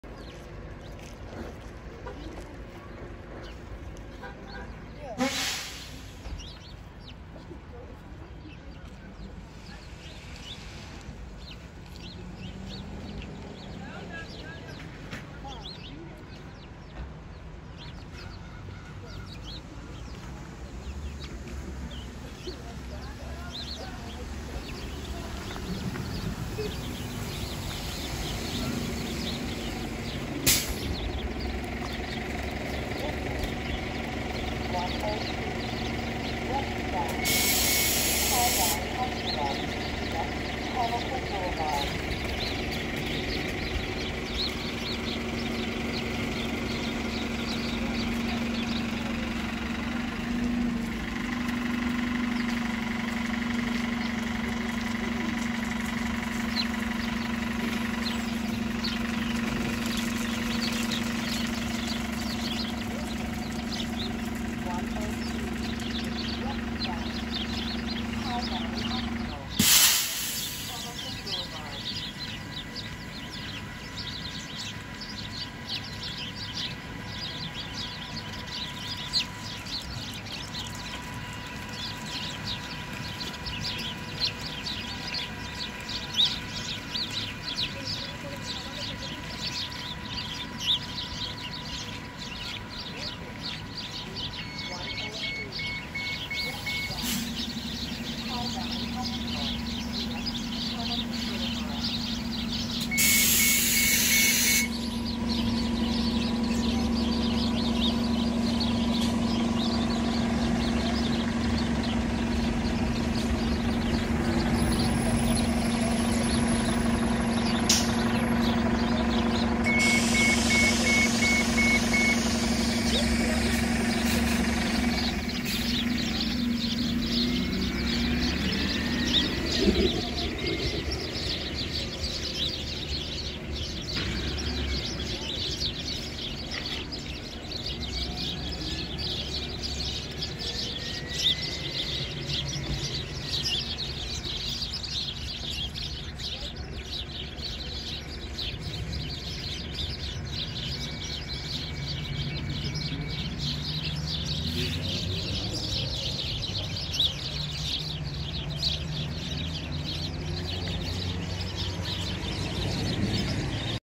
Birds chirping in the snow at the Allentown Transit Center
Birds taking shelter in a tree at the Allentown Transit Center in downtown Allentown near the Artswalk